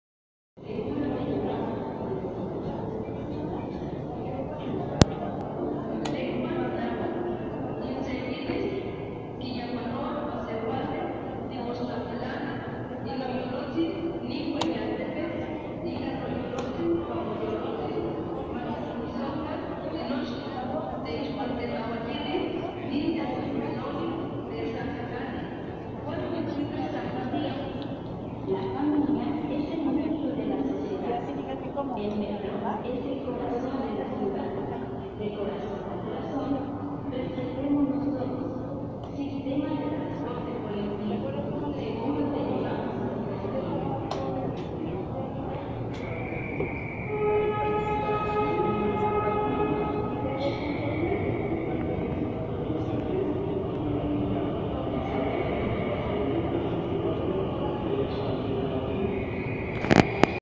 Domingos de ajedrez en el metro.
Sabrán que desde hace algunos meses, se vienen realizando torneos de ajedrez en el metro de la CDMX, igualmente hay clases y partidas amistosas.

Las estaciones cede son: Zapata, Chabacano, La Raza, Ermita y Mixcoac. Todos los domingos a partir de las 10:00 am hasta las 14:00 hrs.